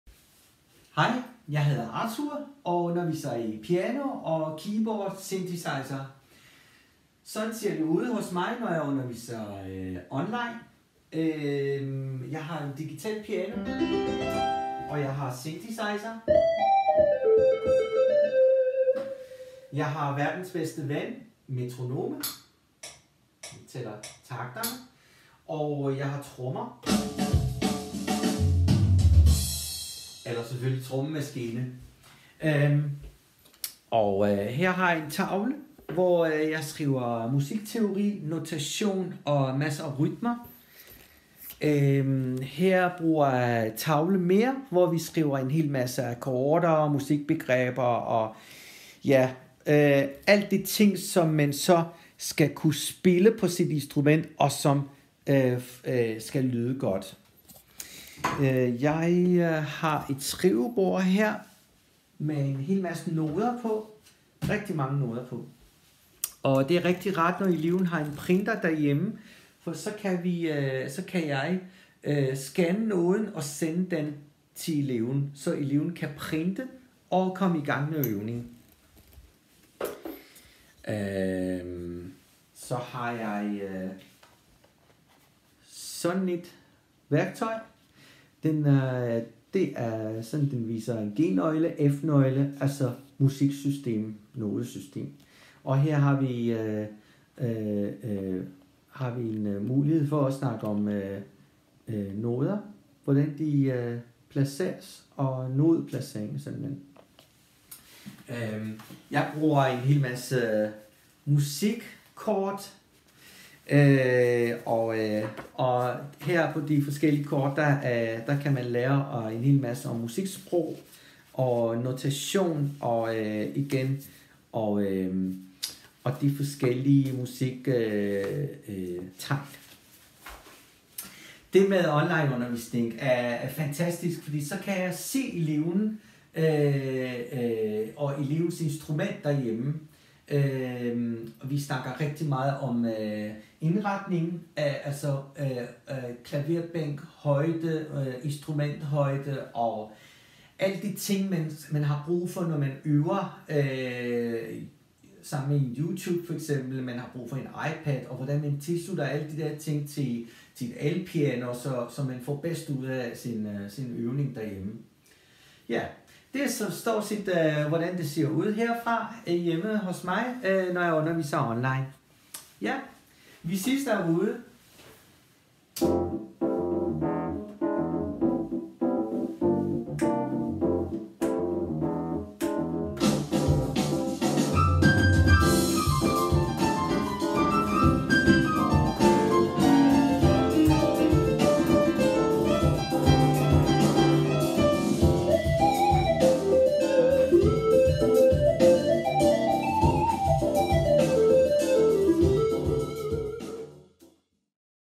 Arthur - Online klaverundervisning
Hvordan er det egentlig at undervise i klaver, når det hele er online? Og er der overhovedet nogle fordele ved? Det fortæller Arthur om i videoen her.